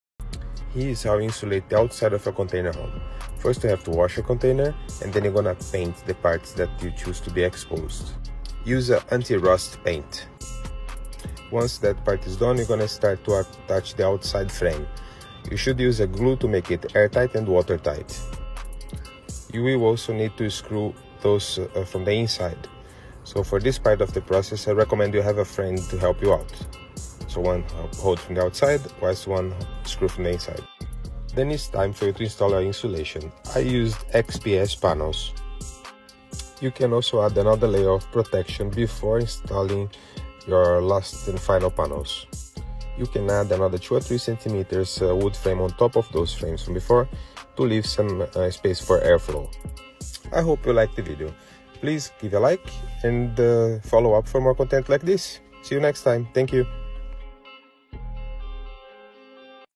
How to Insulate Your Shipping Container from the Outside | Full Step-by-Step Guide
If you’re building a shipping container home, office, or storage unit, one of the most important steps is insulating your container from the outside.

In this guide, we cover:

✅ Why external insulation is better than internal
✅ Best materials: spray foam, rigid boards, mineral wool & fiberglass
✅ Cost considerations & finishing options

Make your container comfortable, durable, and energy-efficient all year round.